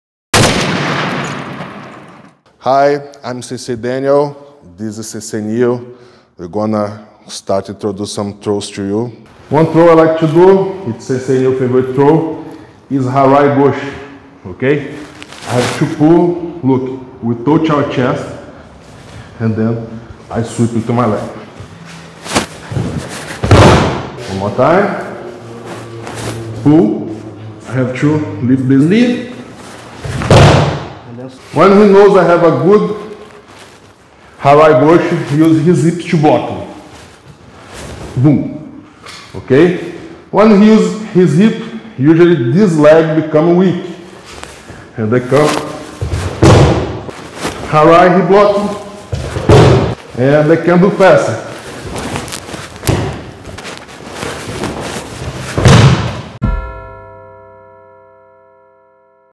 Harai Goshi and Combinations (Renraku Henka Waza)
"Master Harai Goshi: Unlock Powerful Combinations and Renraku Henka Waza Techniques for Judo and BJJ!"

In this comprehensive instructional video, you'll learn the secrets to executing Harai Goshi, a dynamic hip throw in Judo and Brazilian Jiu-Jitsu (BJJ), along with effective combinations and renraku henka waza (sequential variations). With detailed explanations and step-by-step demonstrations, you'll discover how to set up and execute Harai Goshi from various grips, footwork, and body positioning, and how to seamlessly transition to other throws or techniques for maximum effectiveness. Explore advanced strategies, counters, and variations to elevate your throwing game and enhance your grappling skills. Whether you're a Judo or BJJ practitioner looking to level up your stand-up game, this video is a must-watch. Master the art of Harai Goshi and its combinations with this comprehensive instructional video!